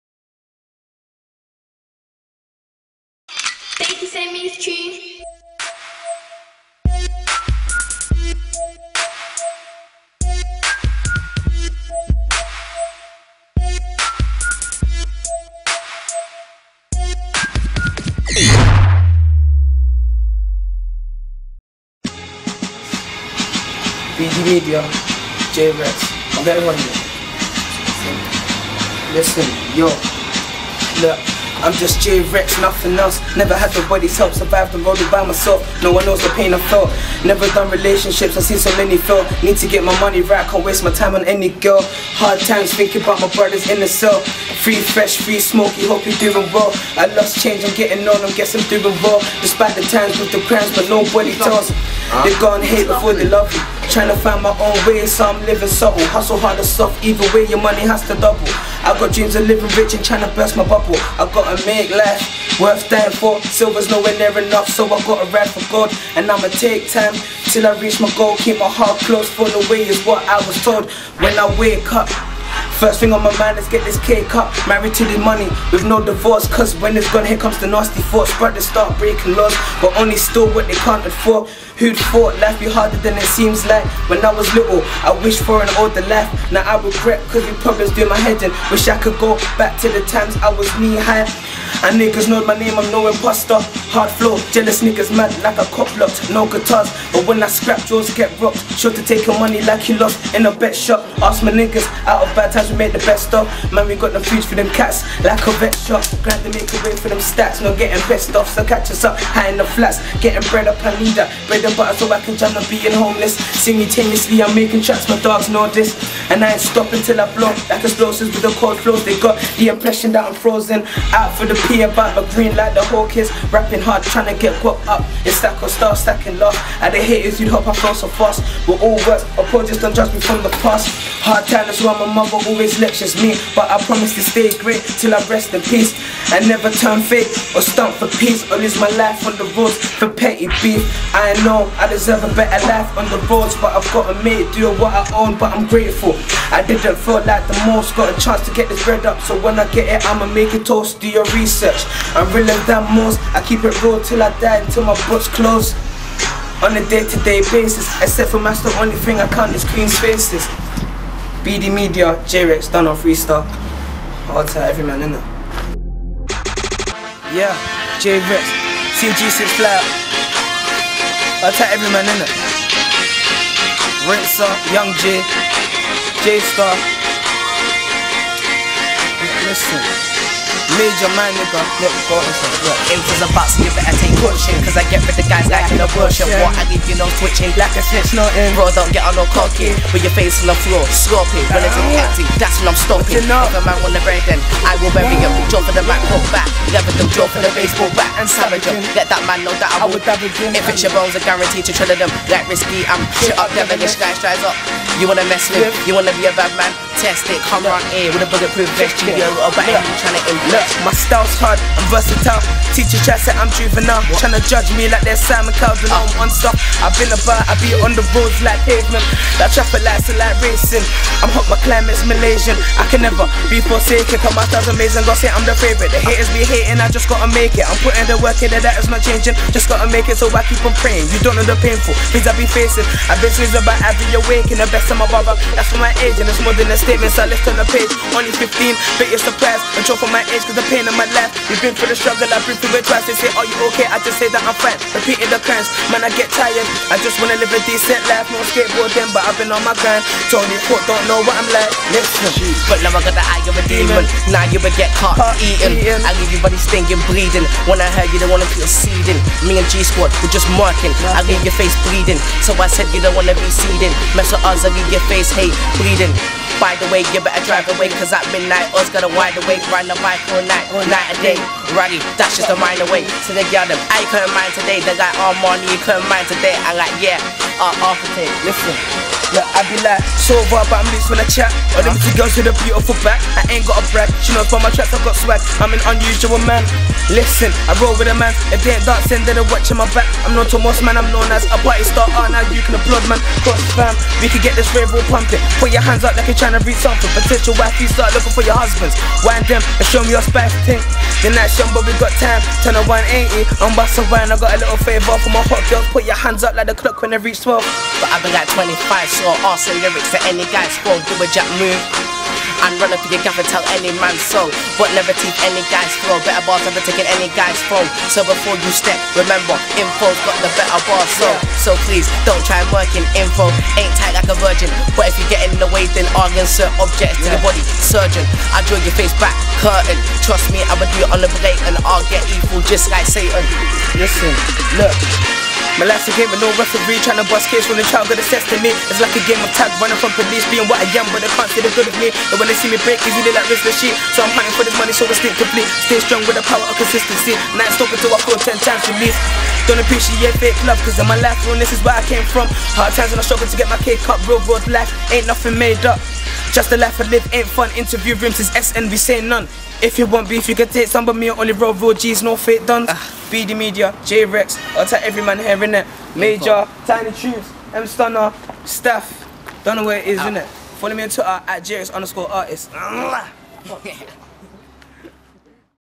BDMEDIA - JREX AND INFO - FREESTYLE
LINKED UP WITH JREX AND INFO. DROPPED ON RAP AND GRIME